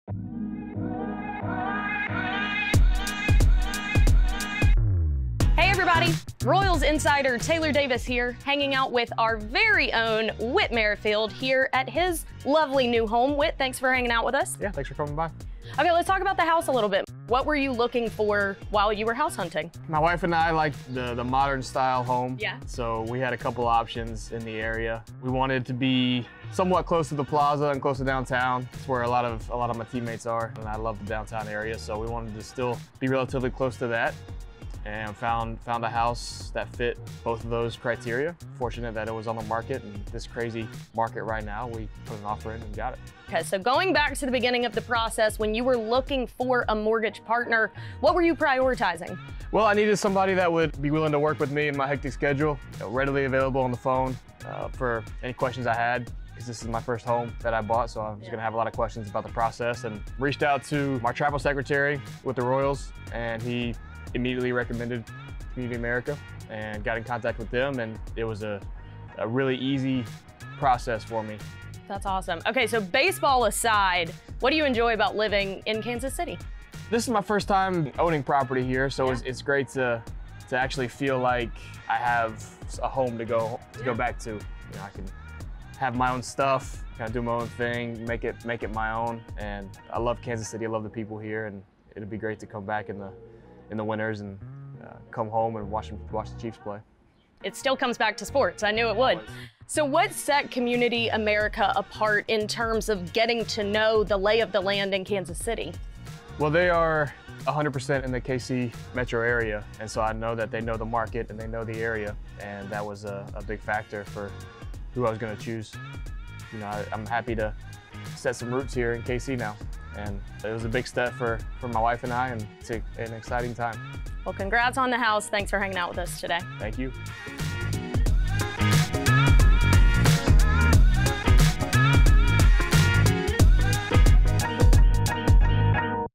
Whit is Officially a Kansas City Homeowner! | Whit's Home Base brought to you by CommunityAmerica
All loans are subject to approval. Whit Merrifield is a paid spokesperson for CommunitAmerica Credit Union.

Whit sits down with the Royals to discuss why he bought a house in Kansas City this season and how CommunityAmerica helped him along the way. "I'm happy to set some roots here in KC now."
Whit's officially a Kansas City homeowner.